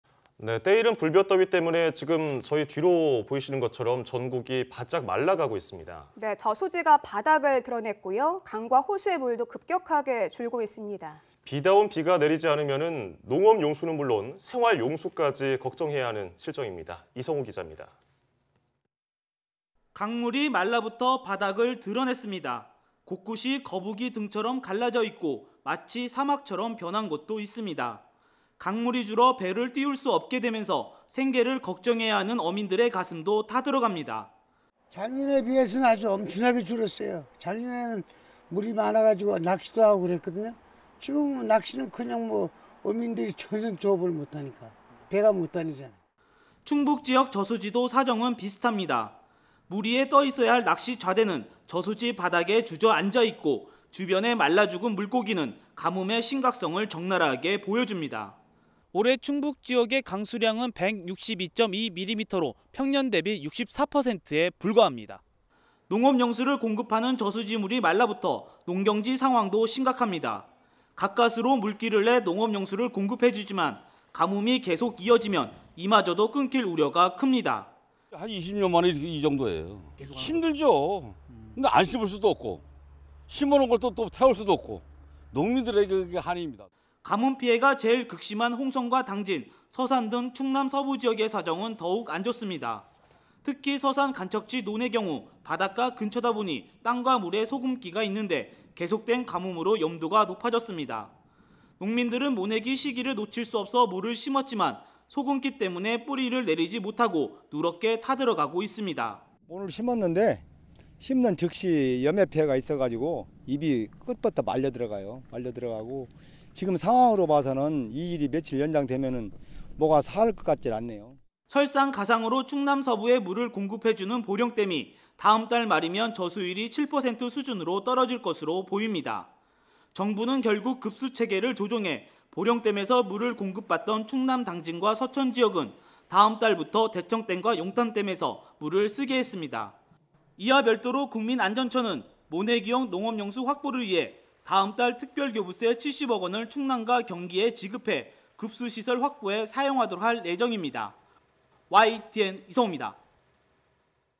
가뭄에 말라버린 강과 저수지...가뭄 피해 극심 / YTN 사이언스
[앵커]
전국이 바짝 말라가고 있습니다.

저수지가 바닥을 드러내고 강과 호수도 급격하게 물이 줄고 있는데요,

가뭄 끝의 단비로는 부족하고, 비다운 비가 내리지 않으면 농업용수는 물론 생활용수도 걱정해야 하는 실정입니다.

이성우 기자입니다.

[기자]
강물이 말라붙어 바닥을 드러냈습니다.

곳곳이 거북이 등처럼 갈라져 있고 마치 사막처럼 변한 곳도 있습니다.

강물이 줄어 배를 띄울 수 없게 되면서 생계를 걱정해야 하는 어민들의 가슴도 타들어 갑니다.

[박재근 / 소양호 어민 : 작년에 비해서 엄청나게 줄었어요. 작년에는 물이 많아서 낚시도 하고 그랬는데 지금은 낚시는커녕 어민들이 전혀 조업을 못 하니까 배가 못 다니잖아요.]

충북 지역 저수지도 사정은 비슷합니다.

물 위에 떠 있어야 할 낚시 좌대는 저수지 바닥에 주저앉아 있고, 주변의 말라죽은 물고기는 가뭄의 심각성을 적나라하게 보여줍니다.

올해 충북 지역의 강수량은 162.2㎜로 평년대비 64%에 불과합니다.

농업용수를 공급하는 저수지 물이 말라붙어 농경지 상황도 심각합니다.

가까스로 물길을 내 농업용수를 공급해주지만, 가뭄이 계속 이어지면 이마저도 끊길 우려가 큽니다.

[신오식 / 충북 진천군 초평면 : 20년 만에 이 정도에요. 힘들죠. 안 심을 수도 없고 심어 놓은 것을 태울 수도 없고 농민들의 한입니다.]

가뭄 피해가 제일 극심한 홍성과 당진, 서산 등 충남 서부 지역의 사정은 더욱 안 좋습니다.

특히 서산 간척지 논의 경우 바닷가 근처다 보니 땅과 물에 소금기가 있는데 계속된 가뭄으로 염도가 높아졌습니다.

농민들은 모내기 시기를 놓칠 수 없어 모를 심었지만, 소금기 때문에 뿌리를 내리지 못하고 누렇게 타들어 가고 있습니다.

[전춘식 / 충남 서산시 부석면 : 모를 심었는데 심는 즉시 염해 피해가 있어서 잎이 끝부터 말려 들어가요. 지금 상황으로 봐서 이 일이 며칠 연장되면 모가 살 것 같지 않네요.]

설상가상으로 충남 서부에 물을 공급해주는 보령댐이 다음 달 말이면 저수율이 7% 수준으로 떨어질 것으로 보입니다.

정부는 결국 급수 체계를 조정해 보령댐에서 물을 공급받던 충남 당진과 서천지역은 다음 달부터 대청댐과 용담댐에서 물을 쓰게 했습니다.

이와 별도로 국민안전처는 모내기용 농업용수 확보를 위해 다음 달 특별교부세 70억 원을 충남과 경기에 지...